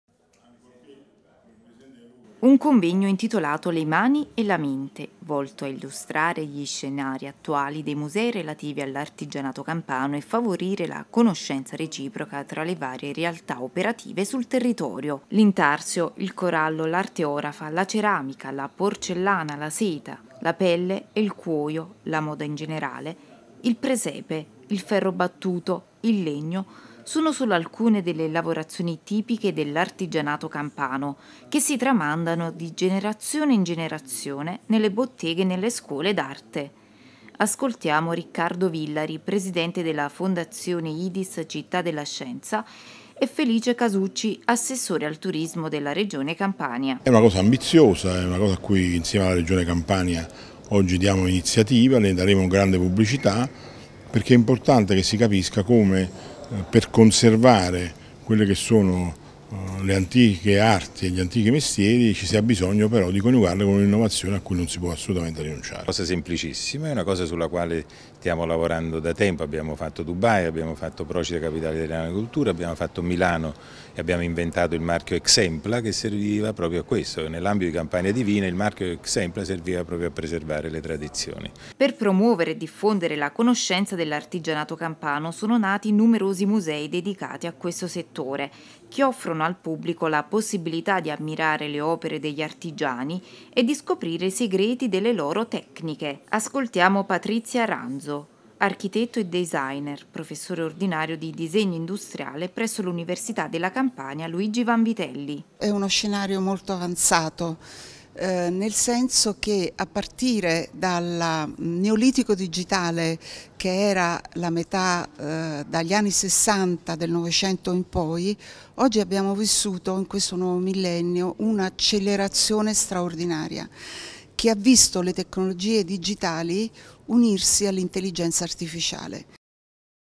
CONVEGNO "LE MANI E LA MENTE" CITTA' DELLA SCIENZA
Un convegno intitolato “Le mani e la Mente” volto a illustrare gli scenari attuali dei musei relativi all’artigianato campano e favorire la conoscenza reciproca tra le varie realtà operative sul territorio. L’intarsio, il corallo, l’arte orafa, la ceramica, la porcellana, la seta, la pelle e il cuoio (la moda in generale), il presepe, il ferro battuto, il legno, sono solo alcune delle lavorazioni tipiche dell’artigianato campano, che si tramandano di generazione in generazione nelle botteghe e nelle scuole d’arte.